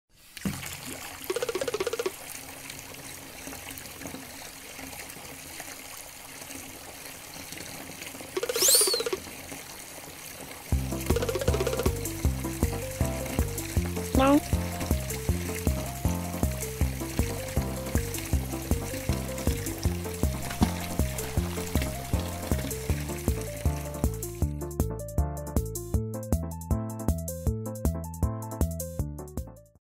fumo Chen bath time
Time for Chen's bath.

Background Music and sounds
funny music and running water for bath and splash
Artist: Alexander